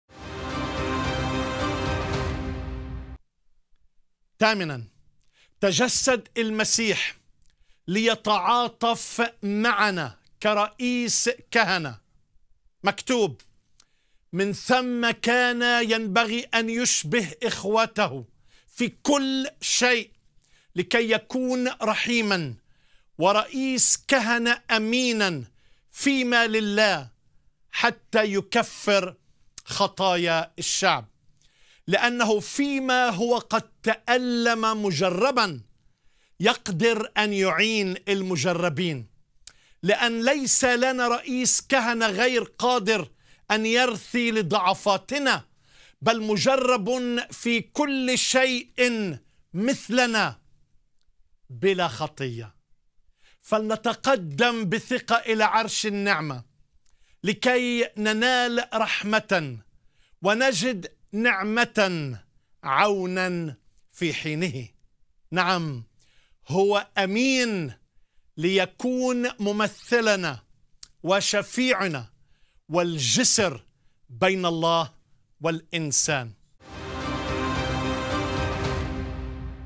530 المسيح رئيس الكهنة .. هل يشعر بنا ويتعاطف معنا؟
عنوان الحلقة: ما_الغرض_من_التجسد؟